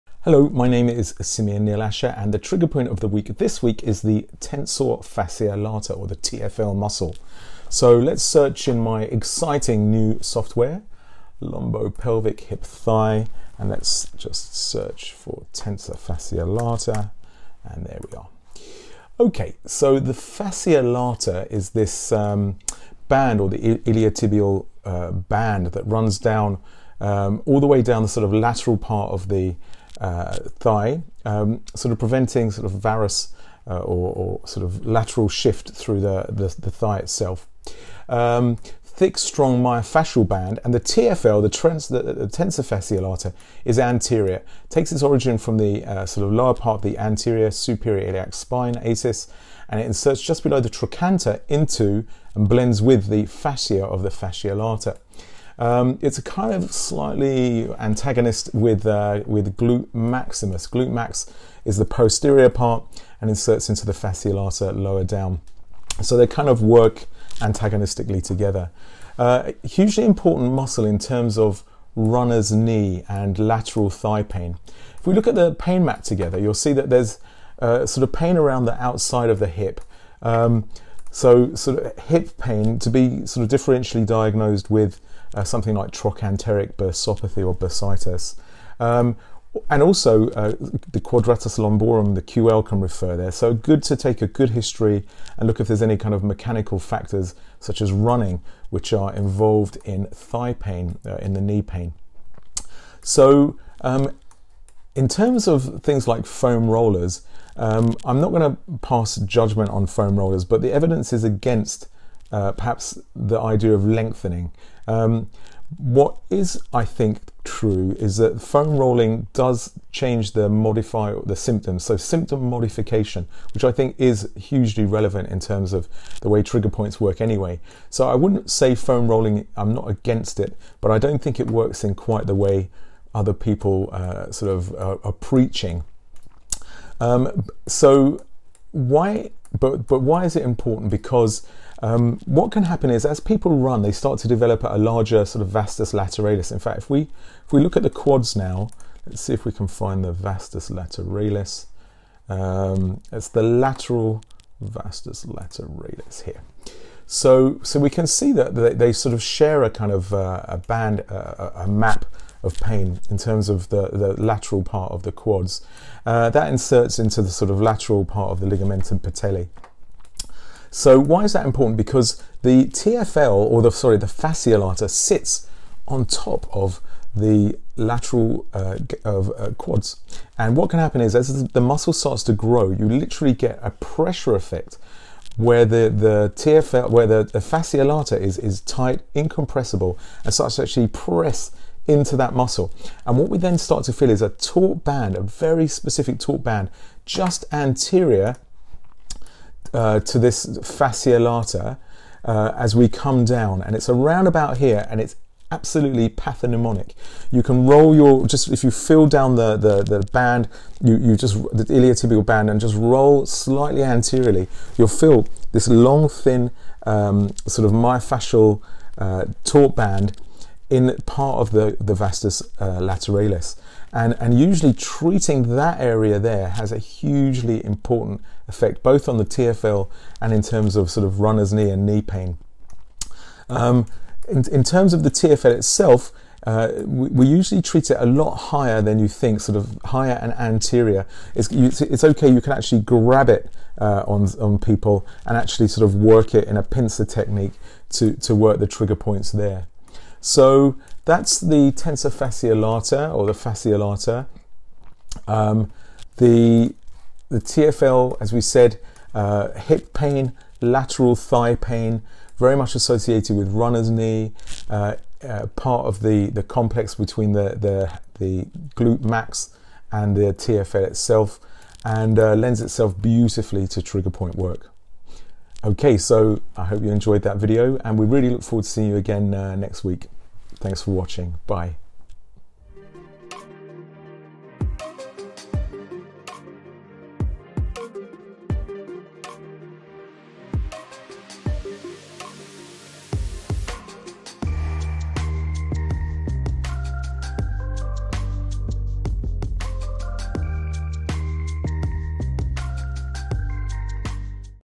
Tensor Fascia Latae - Trigger Point of the Week
The basic functional movement of tensor fasciae latae is walking. The tensor fasciae latae is heavily utilized in horse riding, hurdling and water skiing. Some problems that arise when this muscle is tight or shortened are pelvic imbalances that lead to pain in hips, as well as pain in the lower back and lateral area of knees

Trp Indications
Hip/knee pain (lateral), pain on side lying/fast walking/sitting with knees flexed up, hip-replacement rehabilitation, fracture of neck of femur rehabilitation, a.m. hip stiffness.

TrP Causes
Foot pronation when running (compensating for foot problems), short leg, bursitis of hip, sacroiliac joint dysfunction, poor sit-up technique, climbing, lifting heavy loads, being overweight.

The Gold Standard in Myofascial Medicine, Trigger Points education & practice